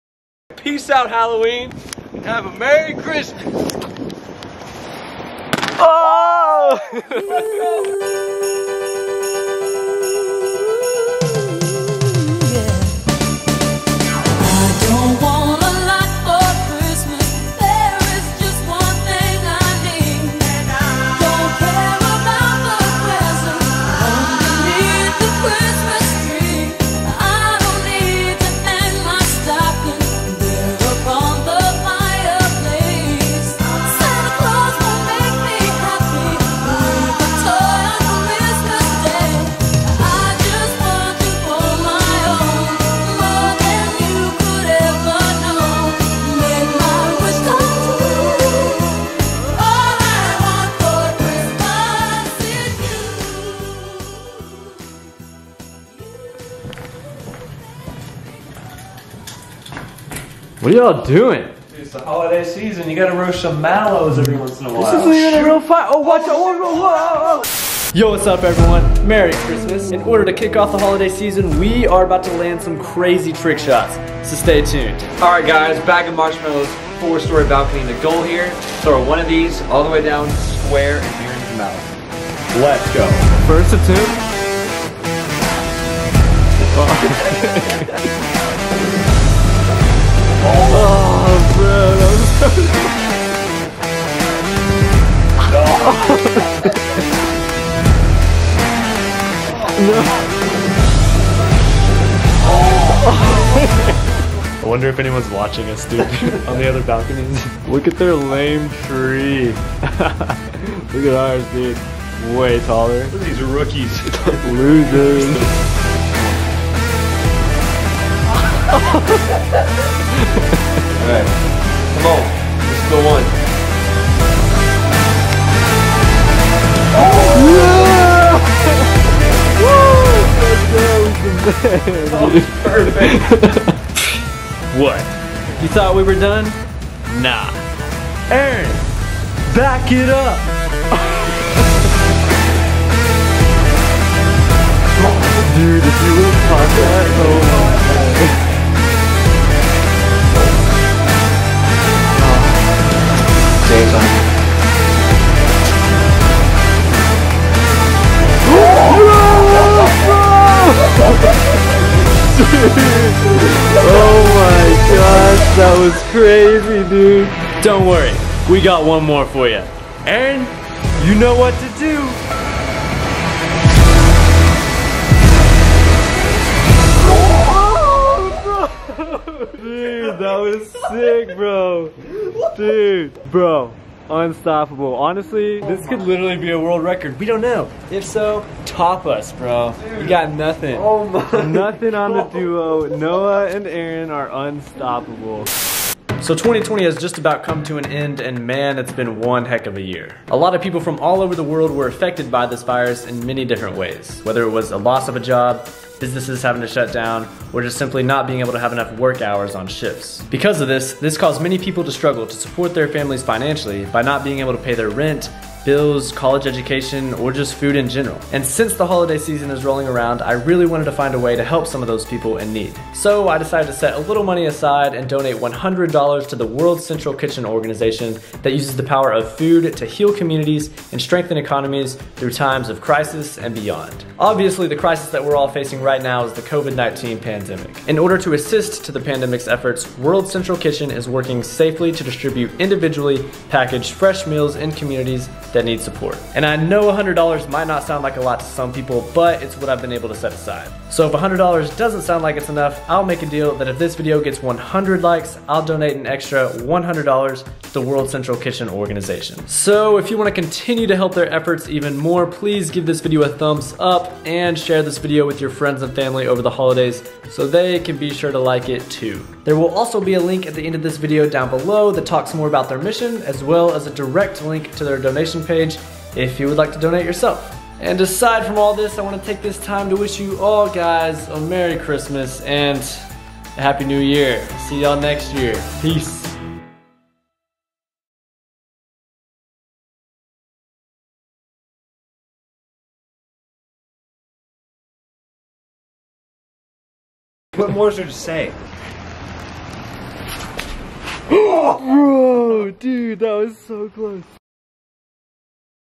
WE BROKE A TRICK-SHOT WORLD RECORD!!! || CHRISTMAS 2020
Jumping into the holiday spirit & landing some CRAZY trick shots!